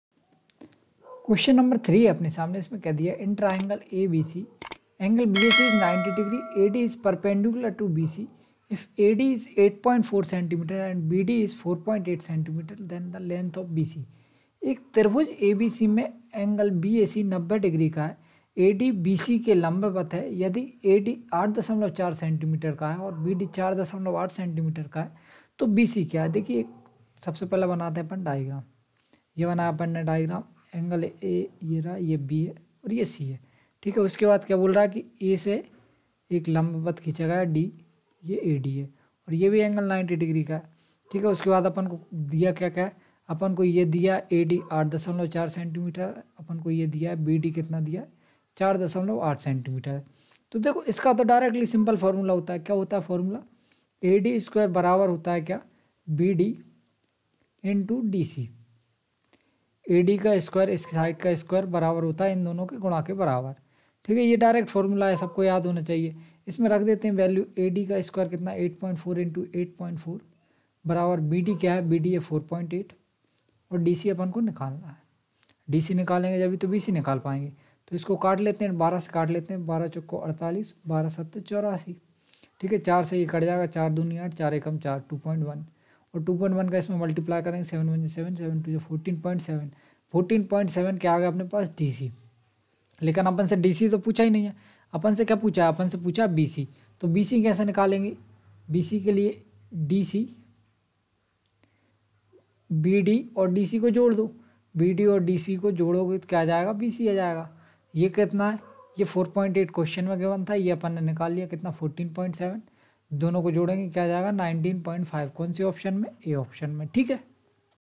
3. In a triangle ABC, ∠𝑩𝑨𝑪 = 𝟗𝟎° and AD is perpendicular to BC. If 𝑨𝑫 = 𝟖.𝟒𝒄𝒎 and 𝑩𝑫 = || edu214
3. In a triangle ABC, ∠𝑩𝑨𝑪 = 𝟗𝟎° and AD is perpendicular to BC. If 𝑨𝑫 = 𝟖.𝟒𝒄𝒎 and 𝑩𝑫 = 𝟒. 𝟖 𝒄𝒎 then the length of BC is:
त्रिभुज ABC में, ∠𝑩𝑨𝑪 = 𝟗𝟎° और AD, BC के लम्बवत है| यदि 𝑨𝑫 = 𝟖. 𝟒 𝒄𝒎 और 𝑩𝑫 = 𝟒. 𝟖 𝒄𝒎 है, तो BC =?
edu214
ram raghuwanshi